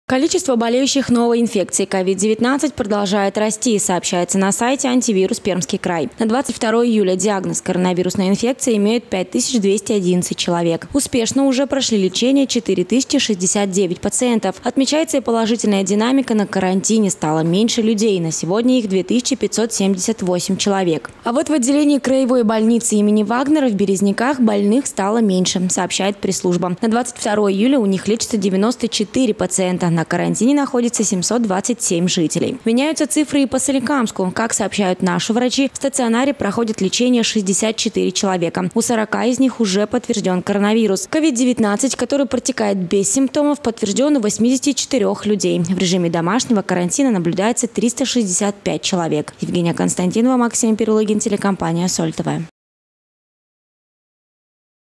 Сводка коронавирус_22.07.2020_СольТВ
Мы продолжаем следить за эпидемической обстановкой в крае и в нашем городе. Как изменилась статистика за сутки? Официальная информация прямо сейчас.